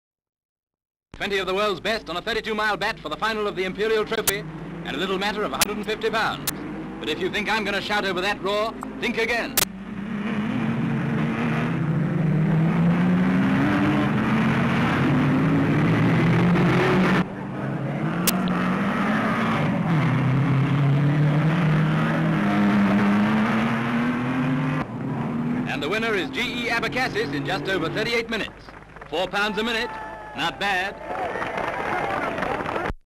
Imperial Trophy - Motor Racing (1938)
No title, paperwork reads - Imperial Trophy motor race.

Crystal Palace, London.

M/S as the racing cars set off, various shots as they speed around the bends, M/S as G.E. (George) Abecassis passes the finish line first. M/S as he is presented with the trophy.

Cataloguer's note: there is an item containing the same material entitled 'Motor racing - Crystal Palace' in 38/81 on *PM0983*.
 FILM ID:983.1